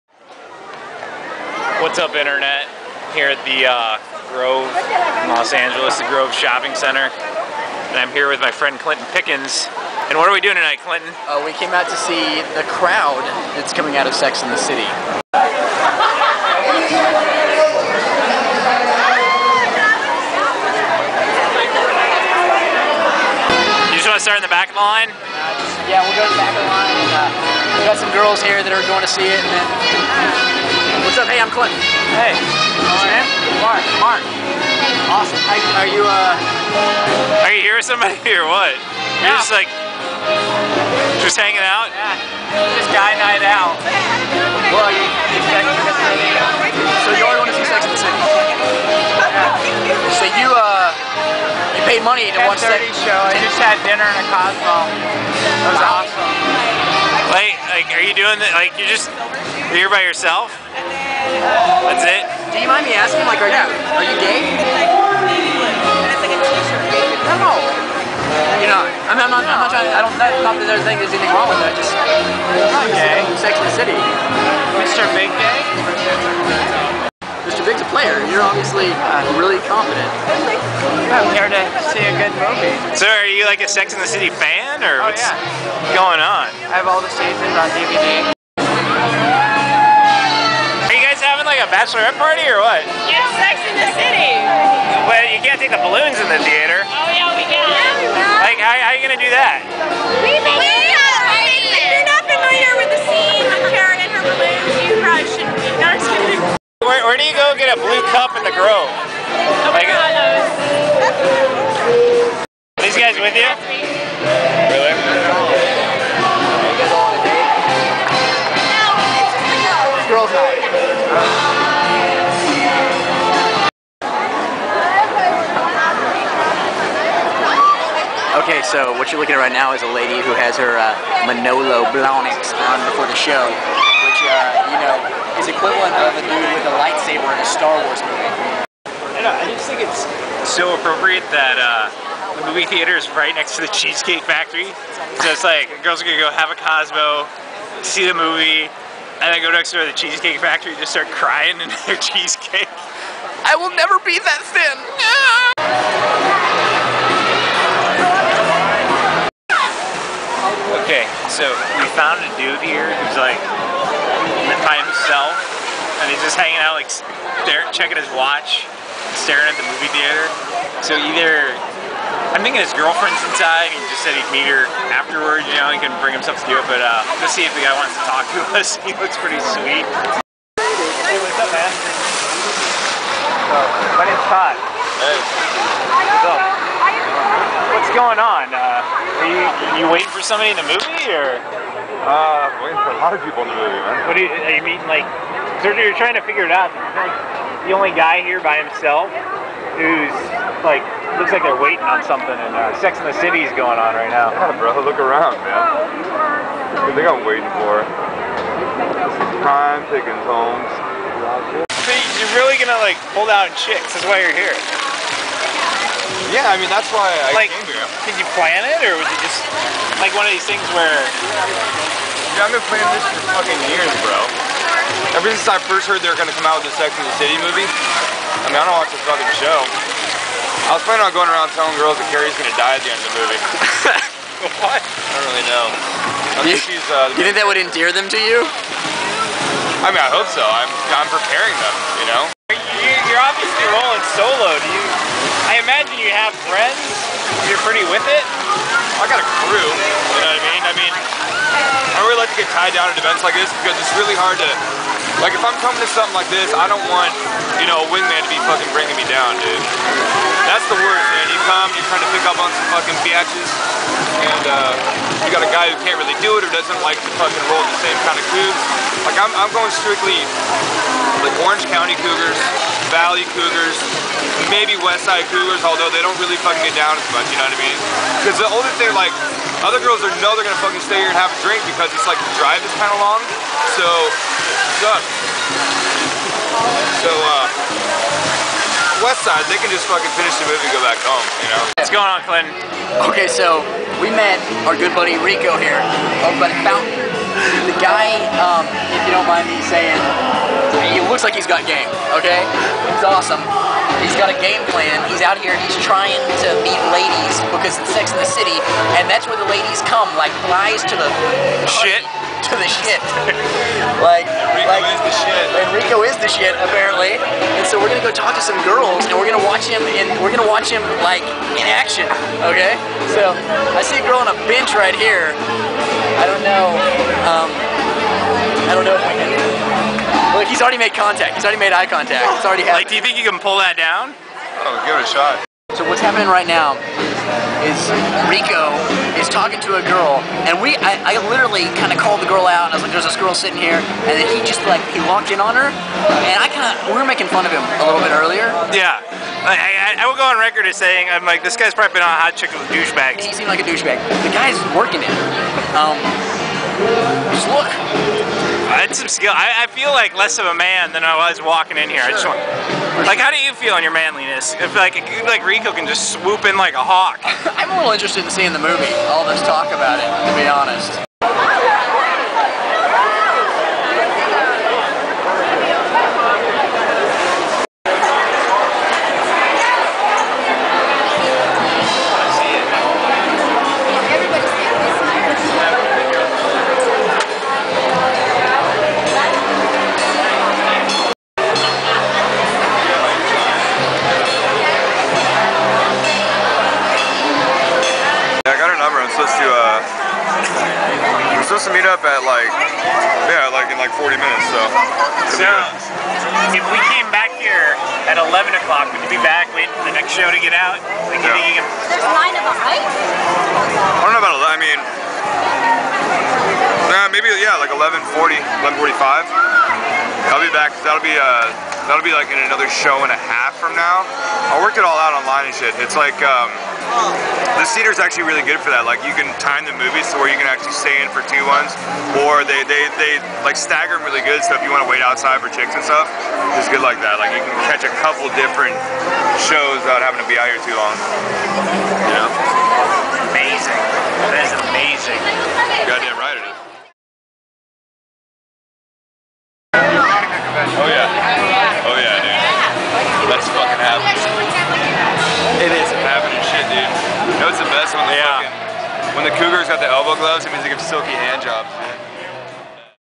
Sex and the City Cougar Hunter vs Drunk Chicks
My good buddy Clinton Pickens and I ventured into the heart of darkness that was crowd for the opening night of Sex and the City.